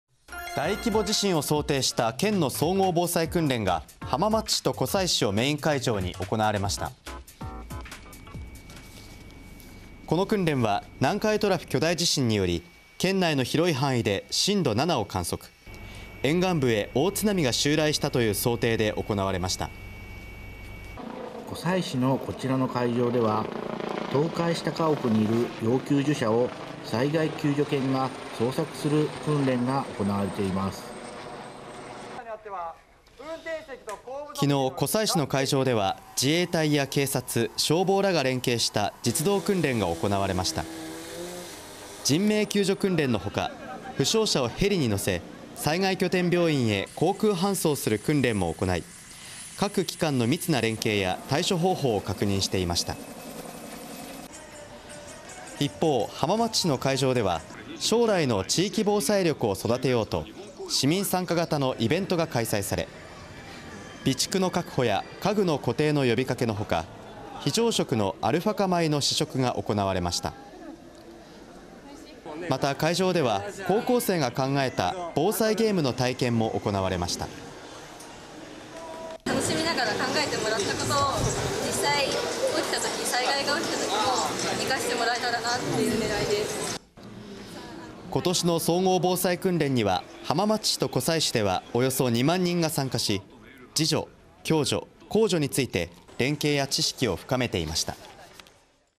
南海トラフ巨大地震に備えて…県総合防災訓練を実施 ３日（静岡県）
大規模地震を想定した静岡県の総合防災訓練が、浜松市と湖西市をメイン会場に行われました。